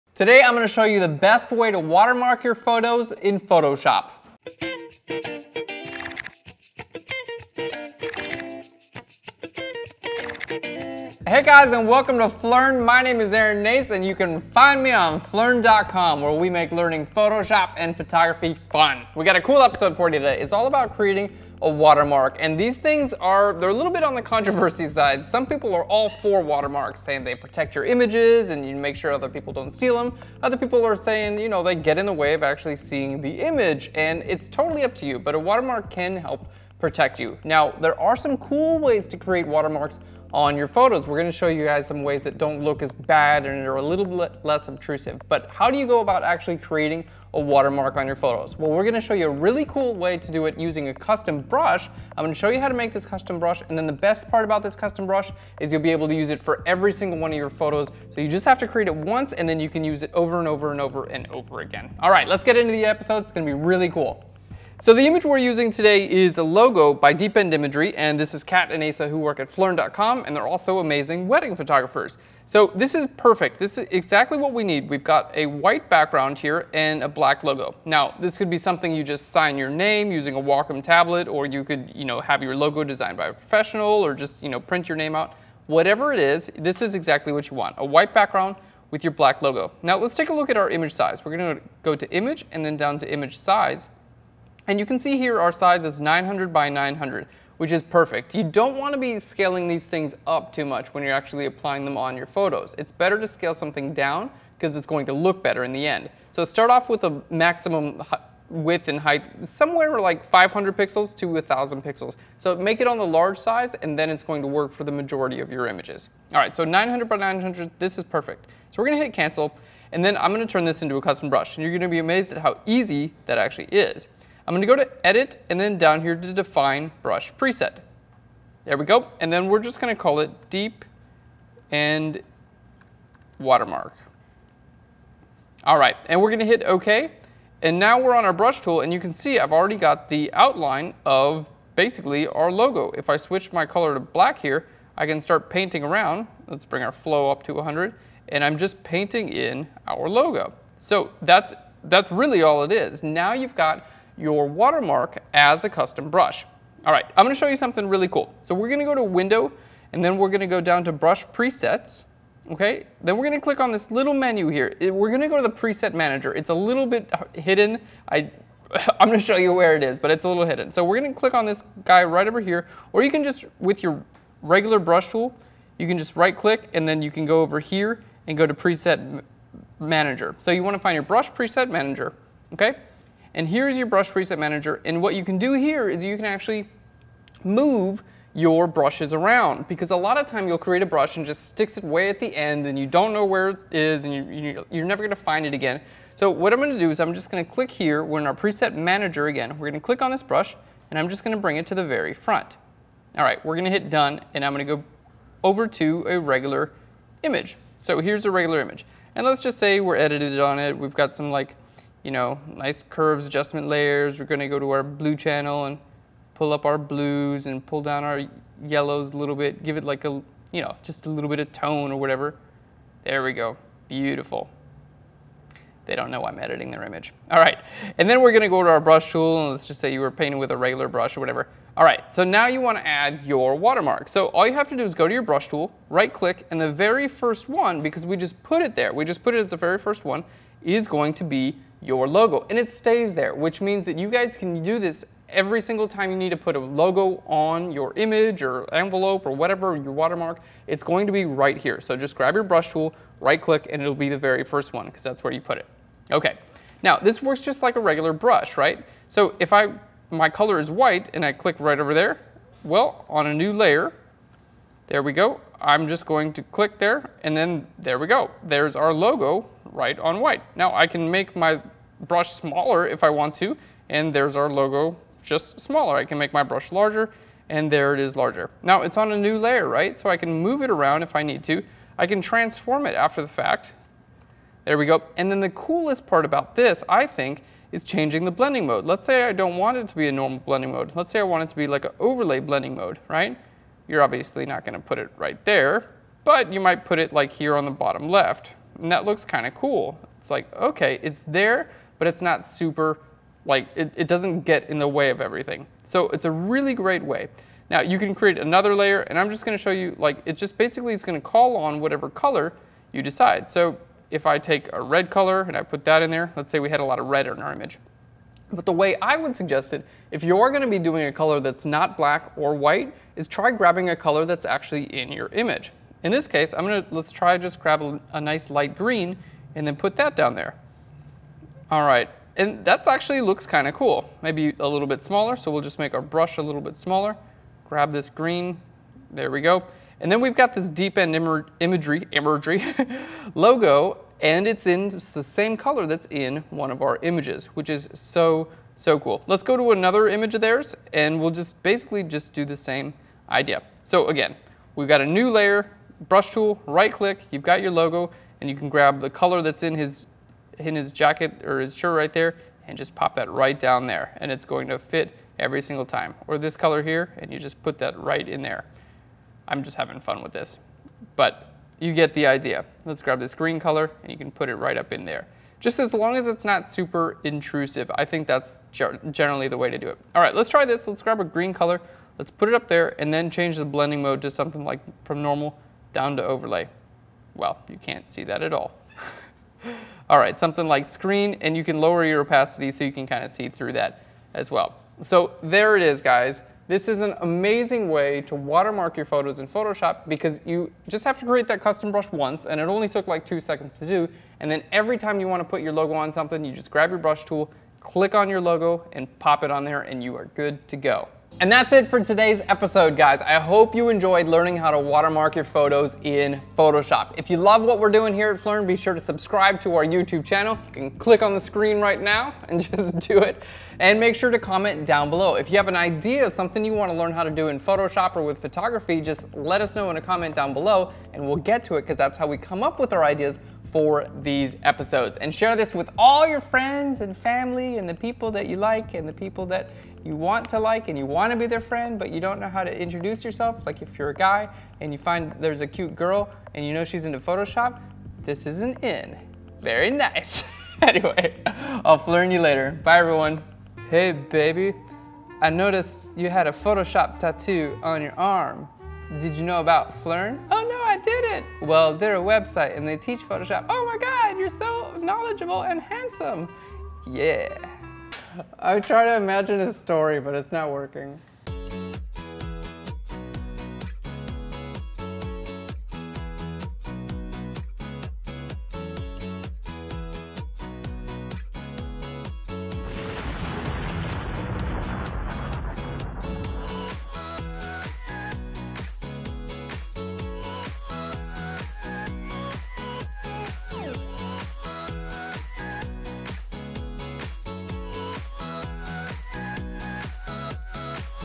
The Best Way to Watermark Your Images in Photoshop
With the rise of blogging and media sharing websites like Pinterest and Tumblr, it has become more and more important to protect your images.  In this episode, we show you an easy way to watermark for photos!

How to Make a Custom Brush of Your Logo

First, place a black version of your logo onto a white background. This logo can be something professionally made, or your signature created using a tablet. Make sure your image size is relatively large, so you can scale it down rather than up and get the best image results. Five hundred to a thousand pixels large works great.

Then, go to 'Edit - Define Brush Preset' and name your brush. Your brush tool will now be in the shape of your logo. Go to your Preset Manager to adjust the order of your brushes and bring this custom brush up to the top of the list.

How to Adjust Your Brush to Add a Watermark

Use the brush tool to paint the logo onto your image. If you create a new layer first, you can transform the size and blending mode of the watermark after the fact. Overlay is a great mode for keeping it subtle.

Then, select a major color from your image and set the watermark to that color in order to further blend it with the image.